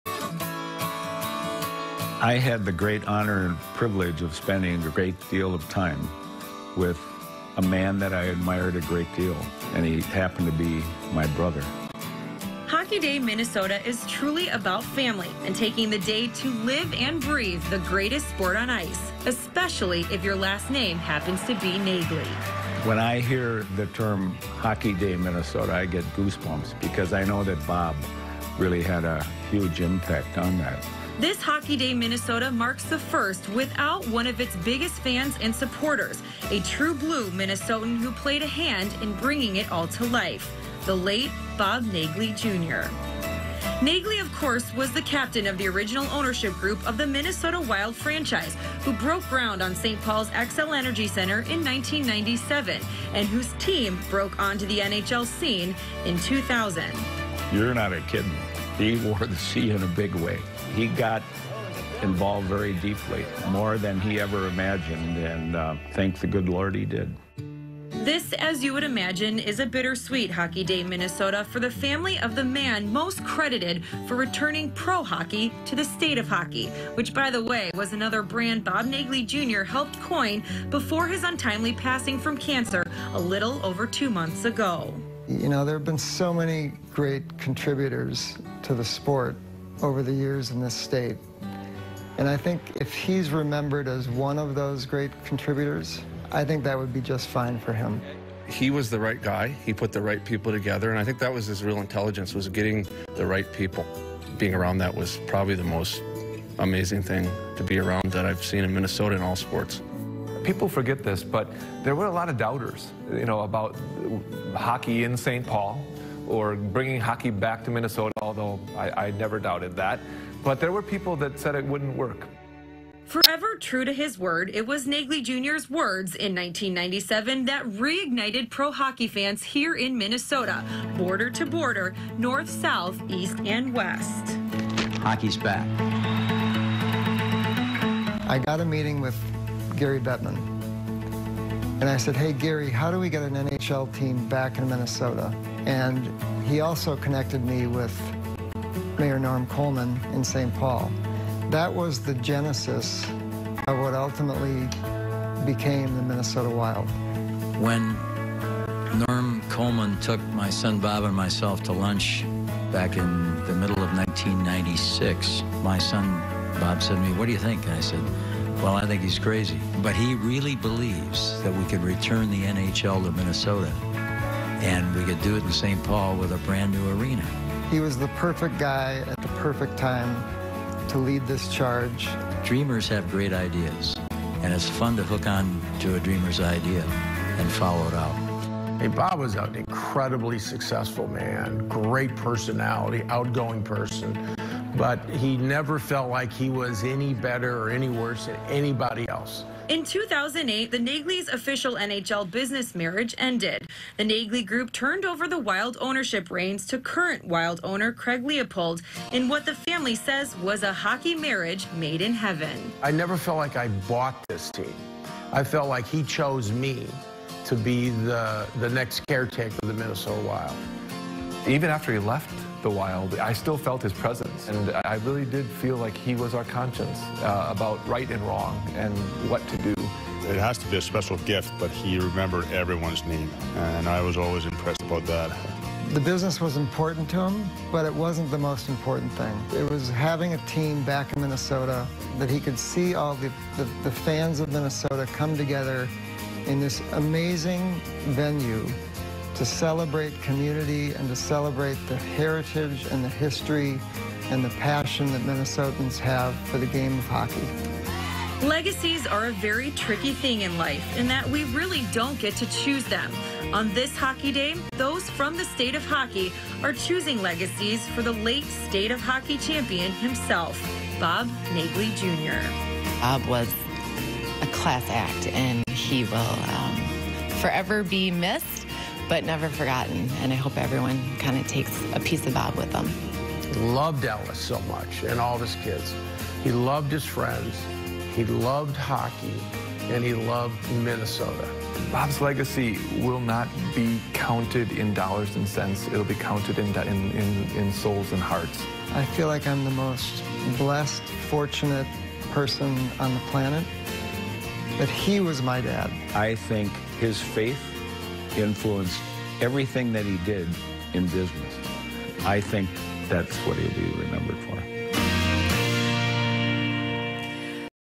HDM 2019: Remembering the late Bob Naegele Jr.
Bob Naegele Jr. transformed the scene of hockey in Minnesota, leading the charge to bring the NHL back to the State of Hockey.